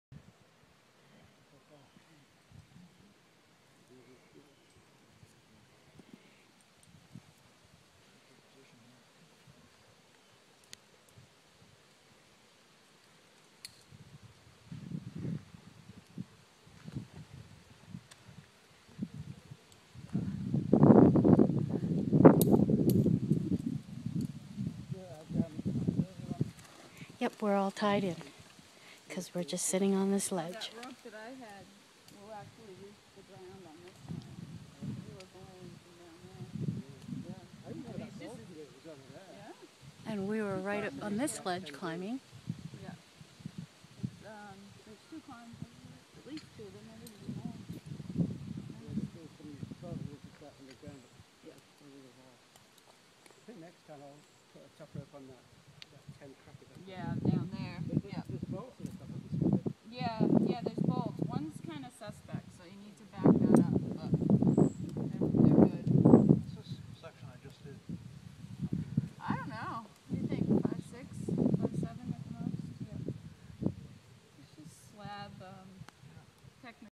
On The Ledge - Rock Climbing Stonehouse Pond State Park, New Hampshire
Waiting on the ledge for a turn at more rock climbing. Stonehouse Pond State Park in Barrington, NH. June 16, 2012. Kellie, Kev, Tom, Jane and Lovena.